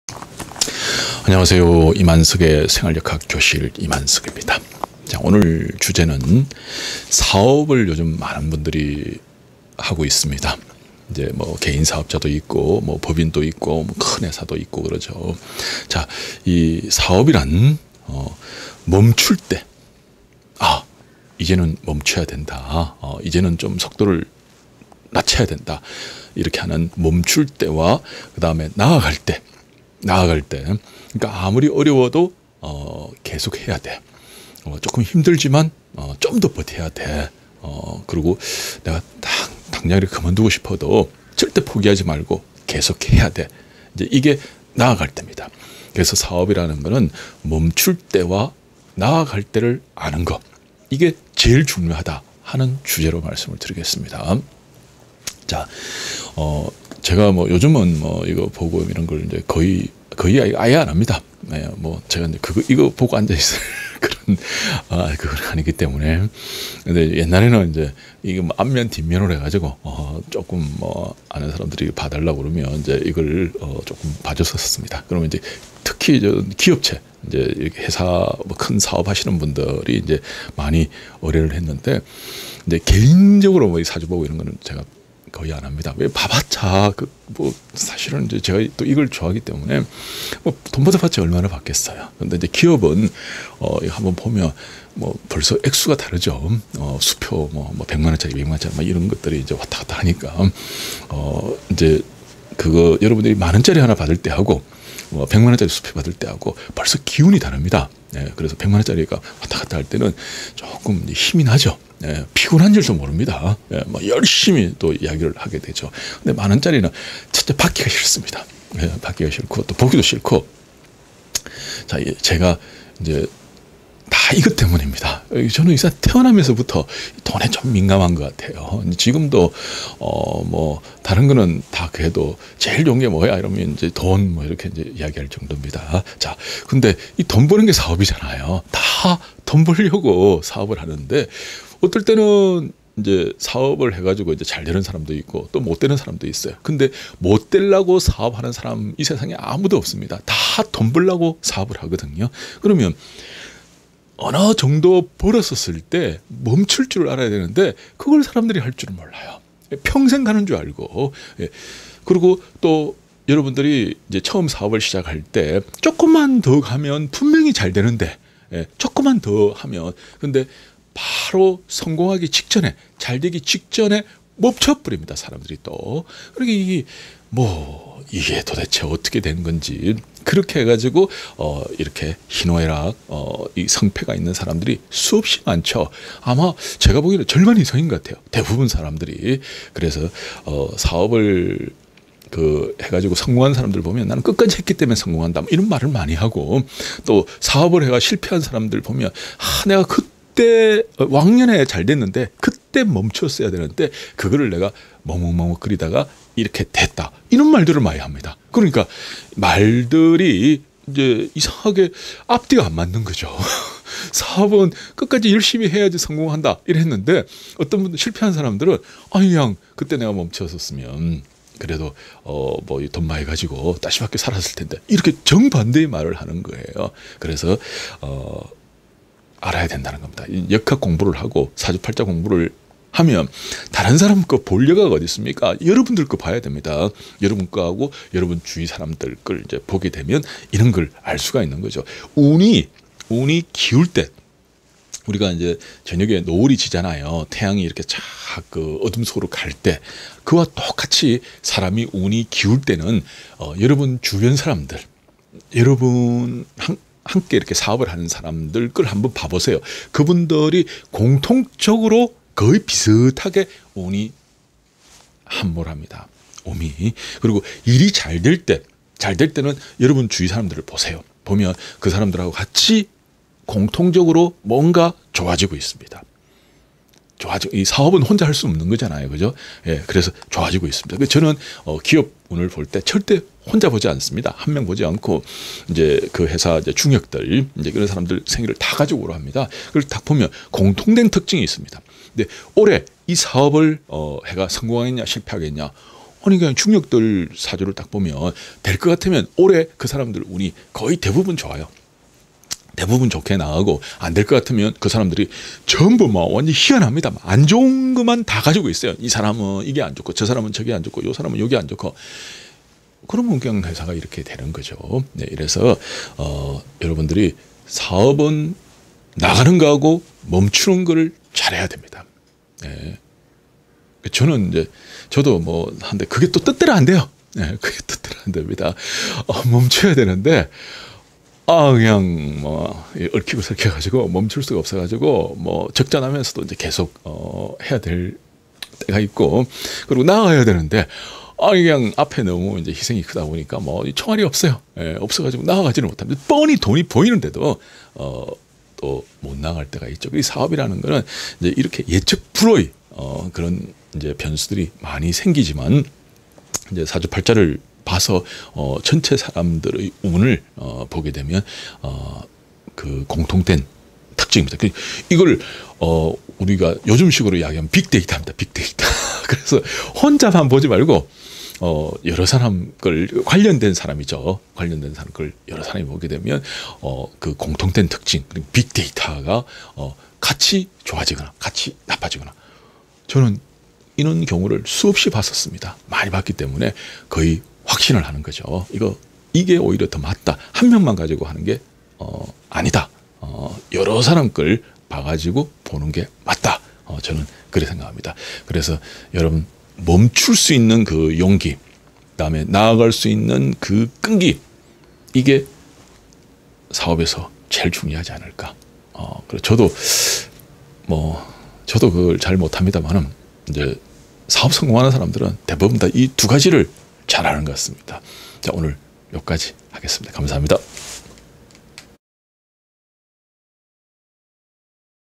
사주팔자 사업에 응용하세요. 멈춤.나아감
사주팔자 사업에 응용하세요.
사업은 흥망성쇠가 분명합니다.
사주팔자를 보면, 도움이 될것입니다 라는 주제로
만든 동영상입니다.